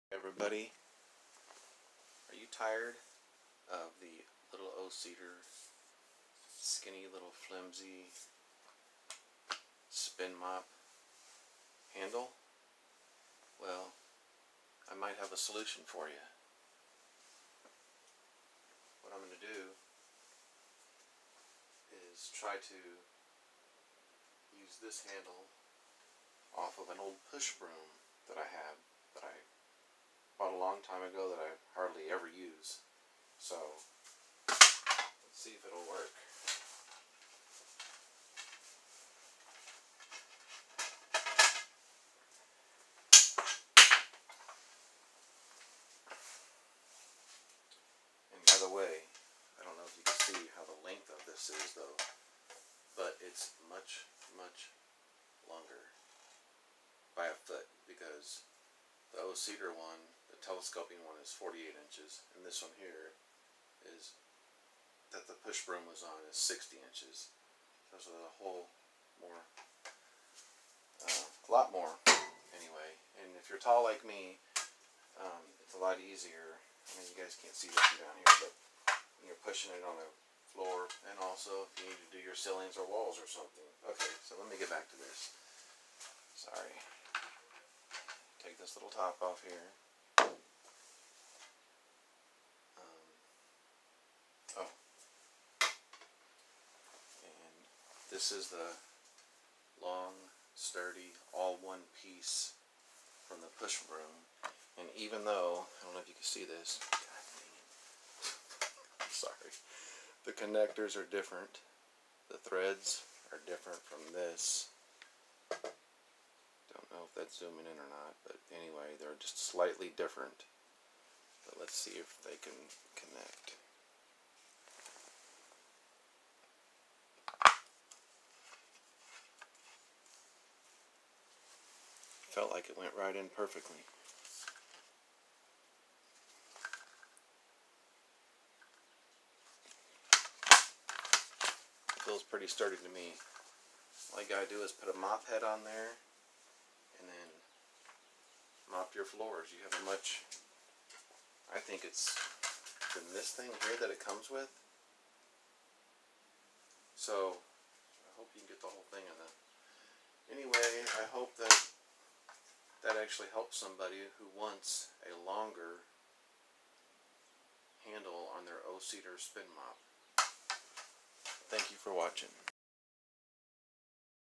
O-Cedar Spin Mop longer handle extension mod 48 Inches to 60 Inches
Quick & Easy way to make your O-Cedar Spin Mop Handle LONGER.